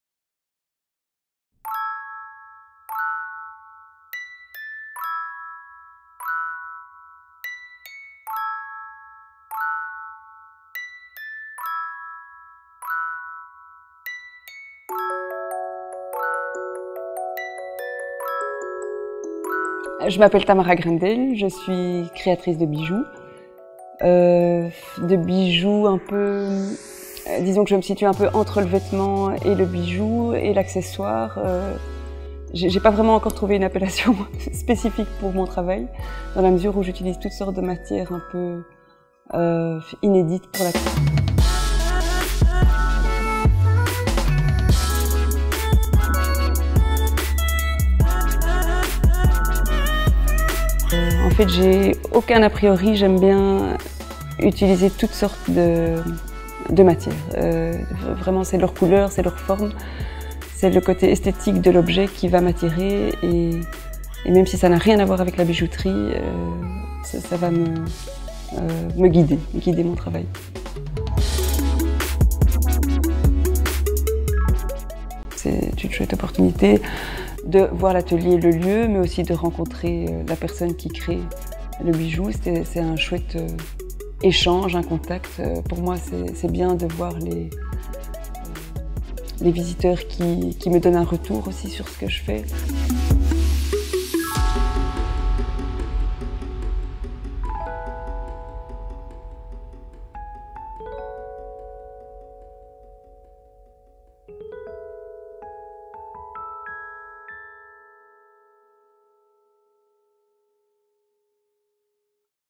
Dag van de Ambachten 2016 - Teaser Tamara Greindl
De Dag van de Ambachten is een jaarlijks evenement dat je voor de 10de (!) keer meevoert naar de ateliers van onze Belgische ambachtslieden. Breng hen gerust een bezoekje op 27 november 2016!